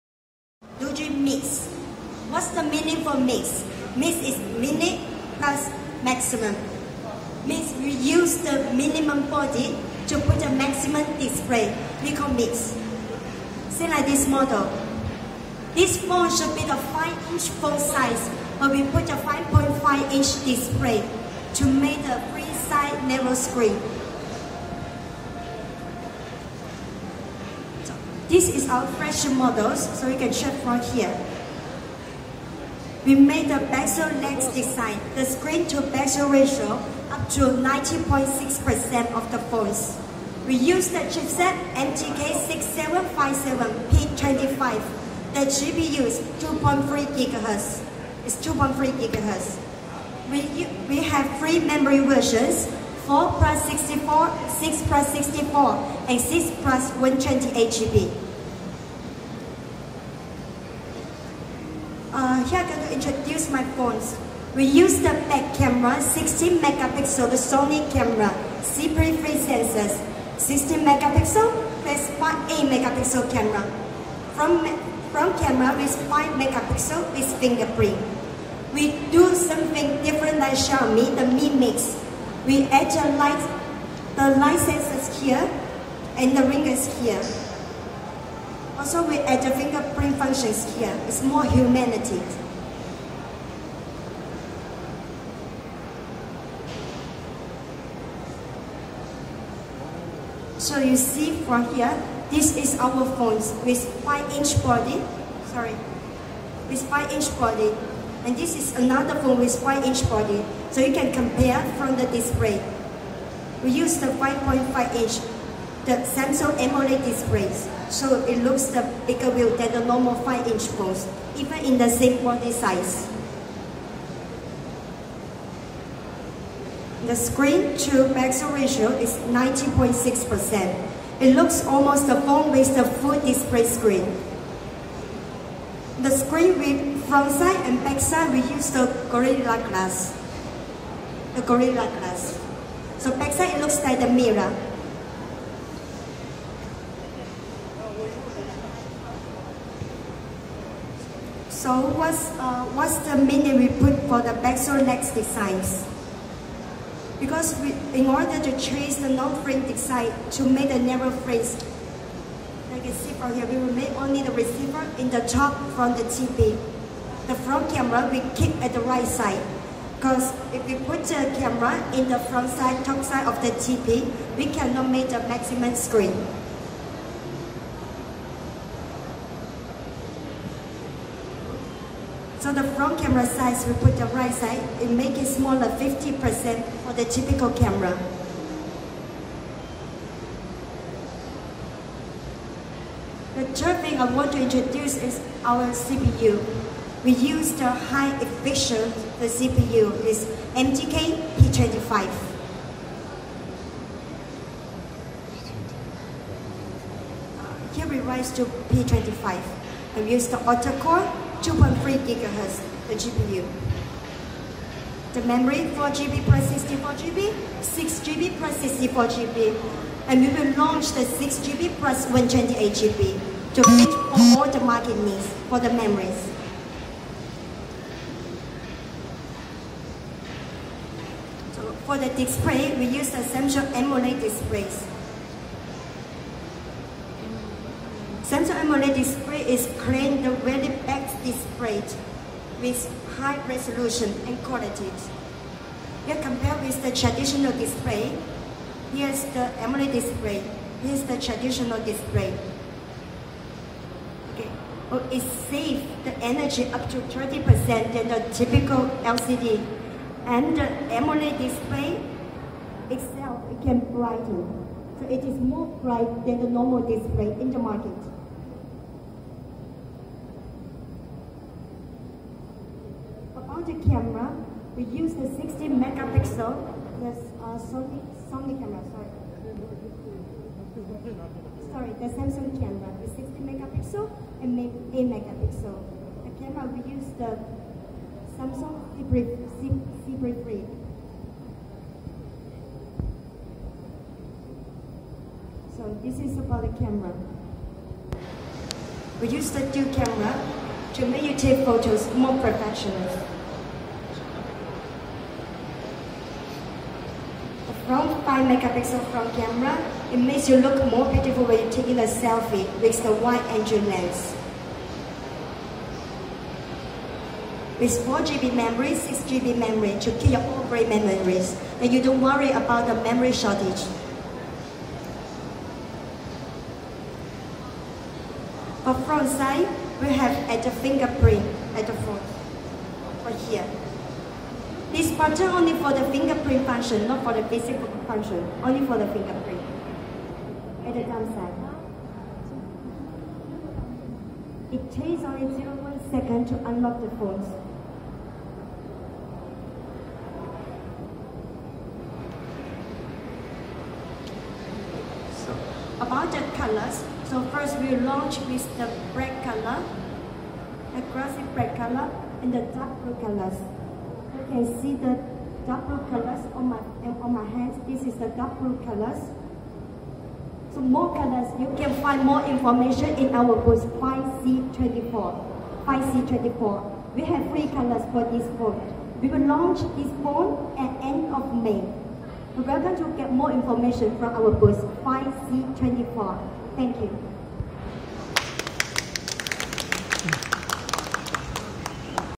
Doogee Mix Release Conference at HK
5.5" AMOLED display in a 5" body.
power saving octa-core CPU.
4GB/6GB LPDDR4x RAM.
64GB/128GB internal storage.
16.0MP+8.0MP dual camera with PDAF.
Upgraded DTouch.